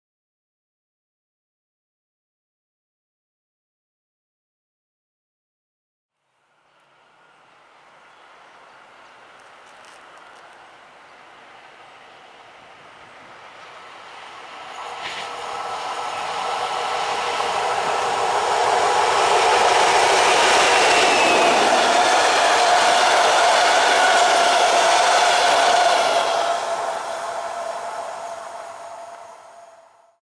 SM42-2486 Captrain z beczkami na kwas octowy do Poznania Franowa.
Zielona SM42-2486 z logiem Captrainu z 6 beczkami do przewozu kwasu octowego ADR 83/2789 mija most nad rzeką Sąsiecznicą w Żmigrodzie i zmierza w kierunku Leszna.

Województwo: Dolnośląskie
Miejscowość: Żmigród
Data: 27 Maj 2021r.
Godzina: 14:05
Przewoźnik: Captrain, Rail-Loco Plata
D29: 271 (Wrocław - Poznań)
Montaż: Camtasia Studio